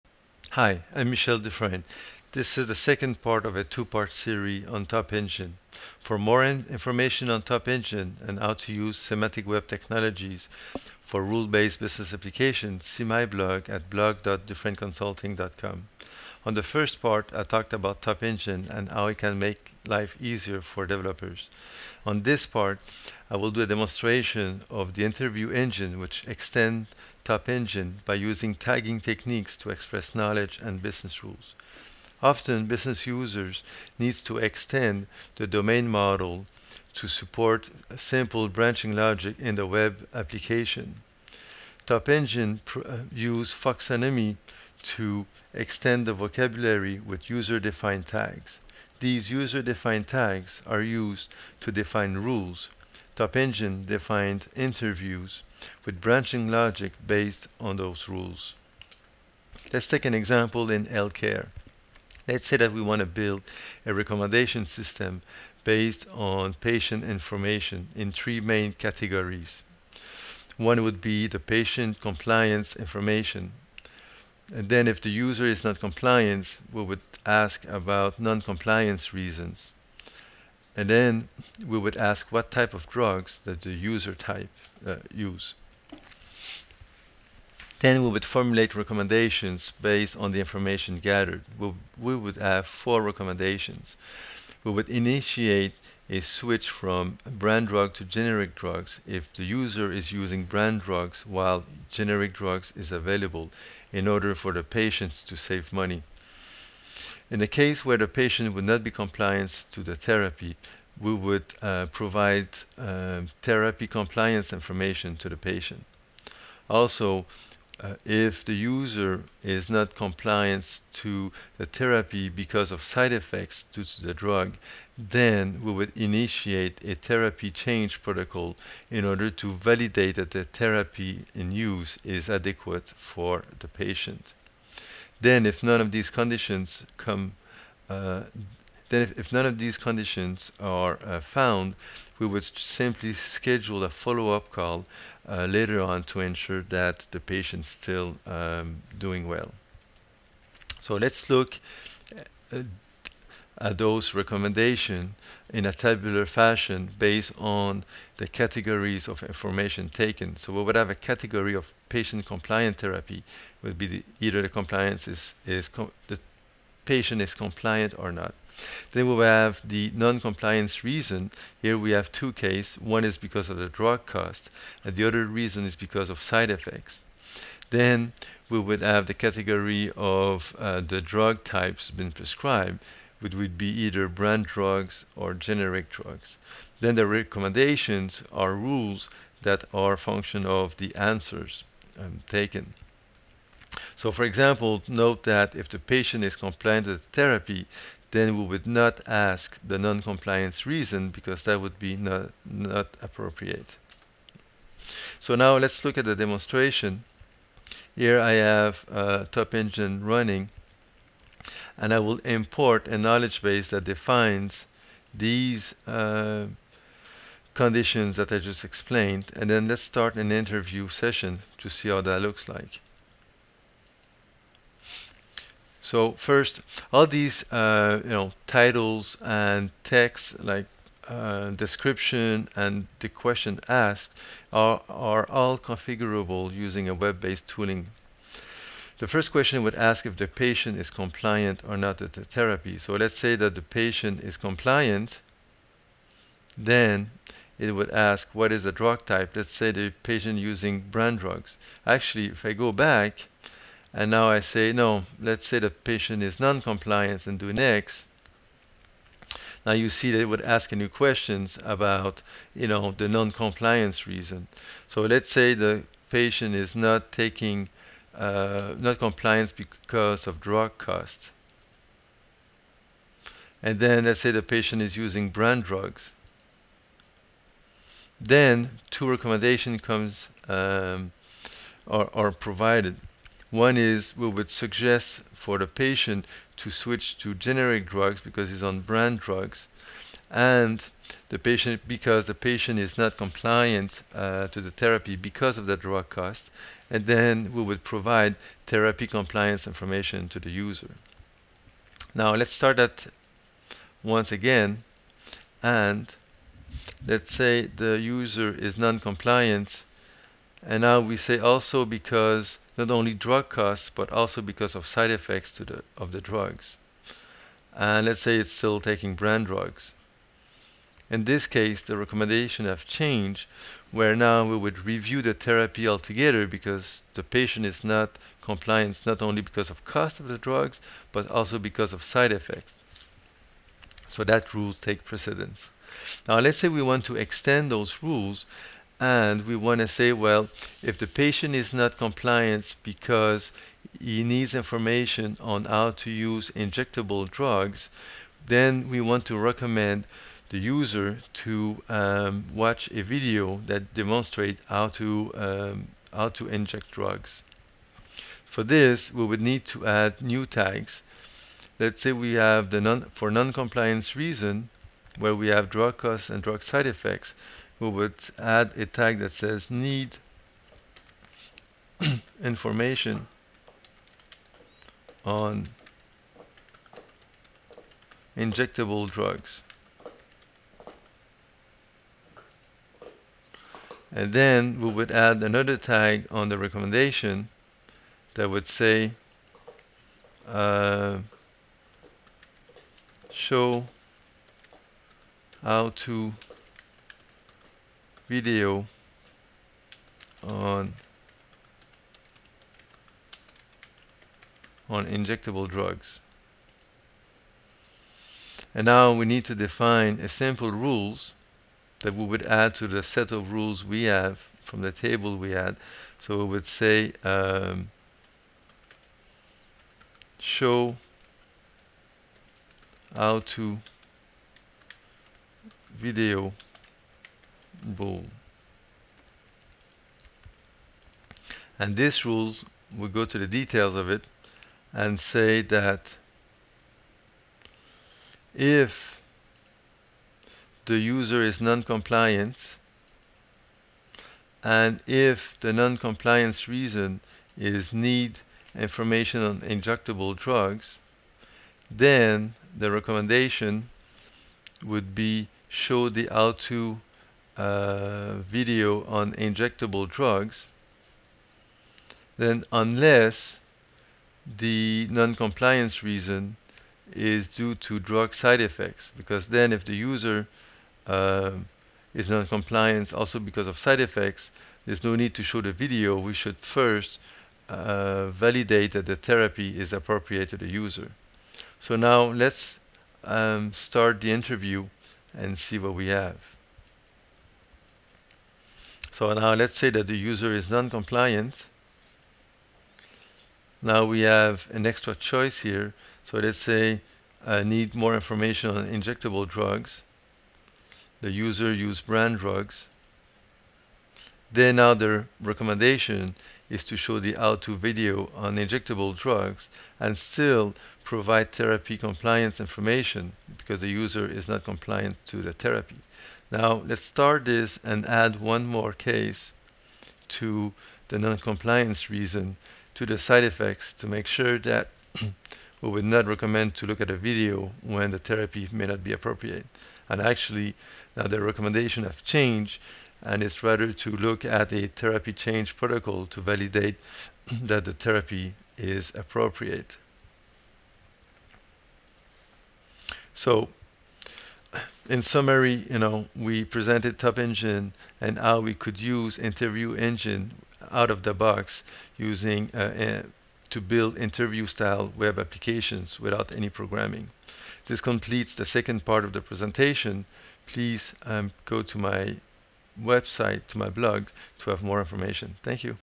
Top Engine - Part 2
Demo Interview Engine to show how folksonomy and tagging techniques can be used to extend a vocabulary with user-defined tags to define rules and branching logic in web-based applications.